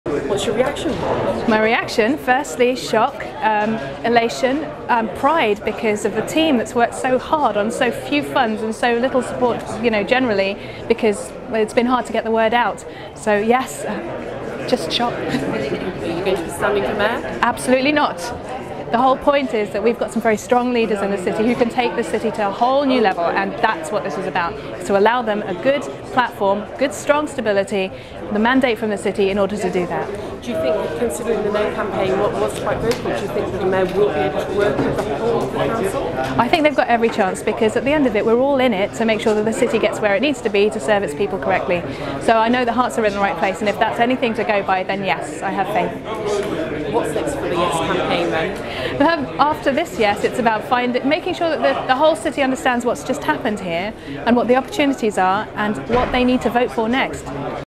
Bristol votes yes for a mayor
Yes campaigner Jaya Chakrabarti gives her initial reaction to the news